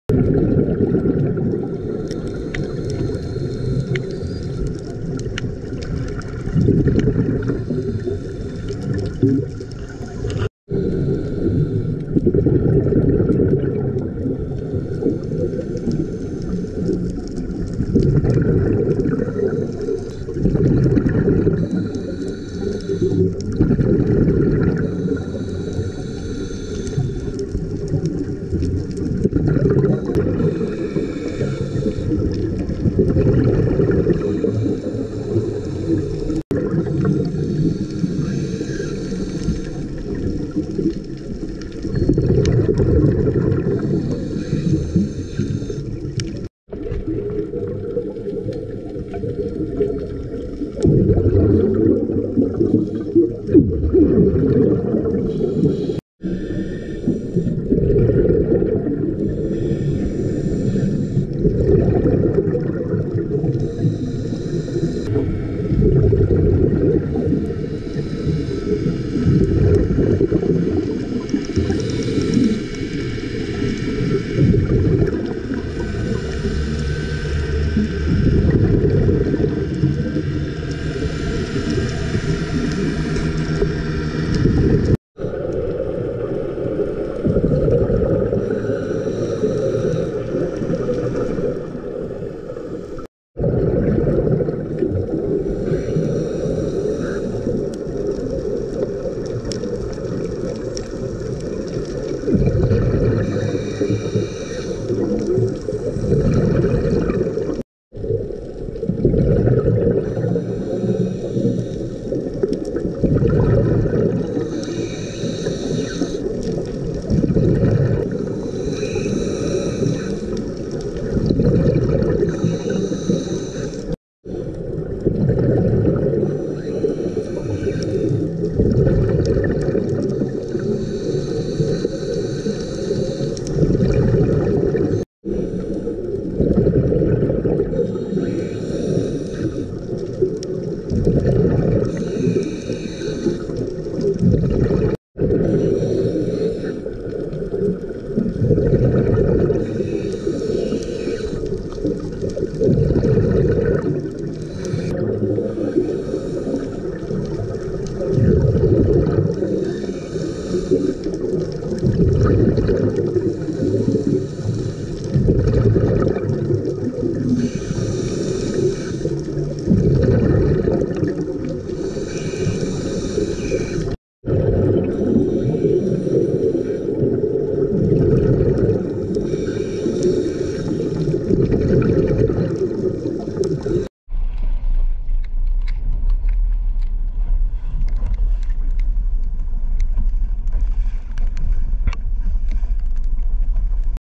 The Best Of Sodwana Bay 2022 Episode Two
Episode Two features the remaining dive sites in Sodwana Bay National Park during June 2022